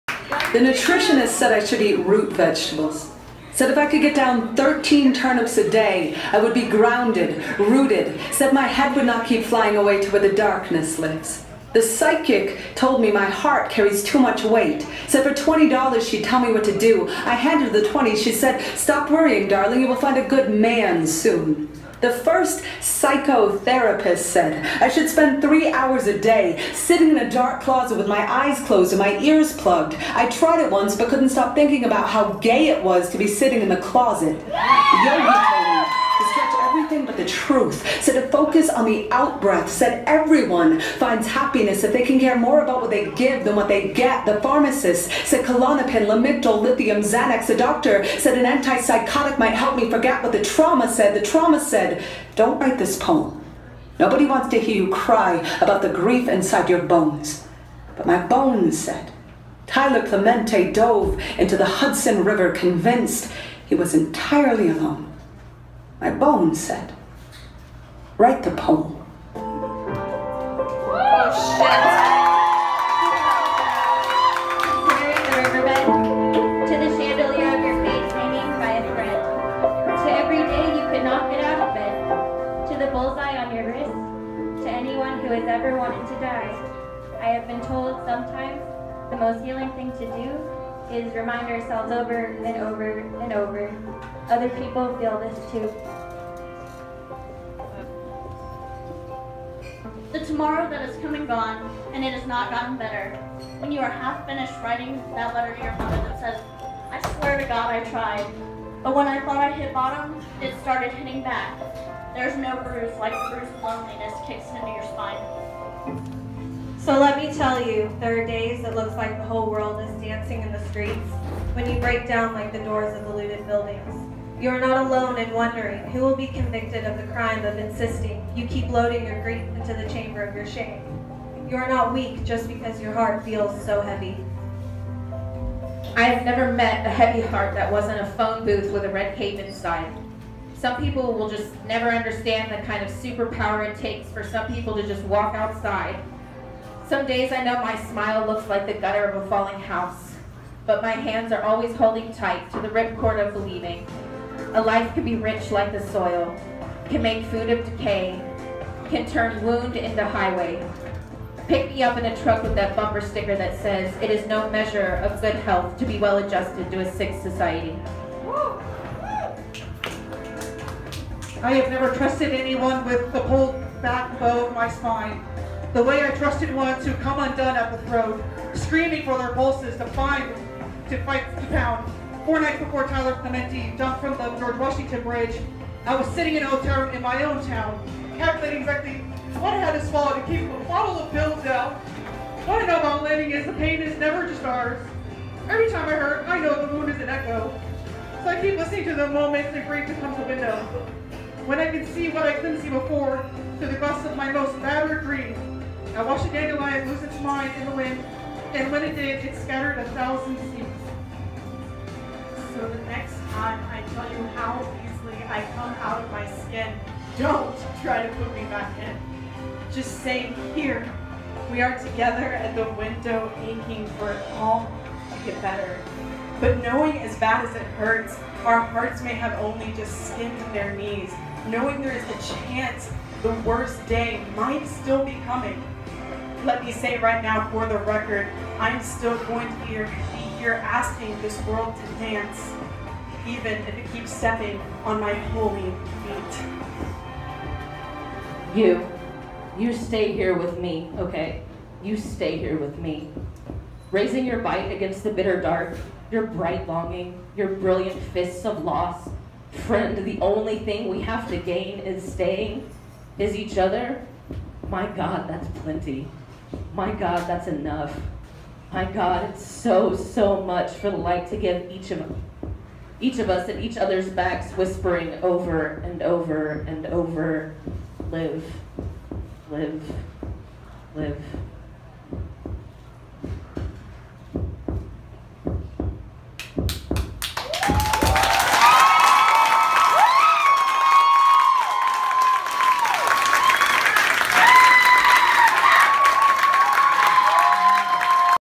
The nutritionist
Performed by Scarlett Frenzy at Glitterbomb in Denton November 17,2016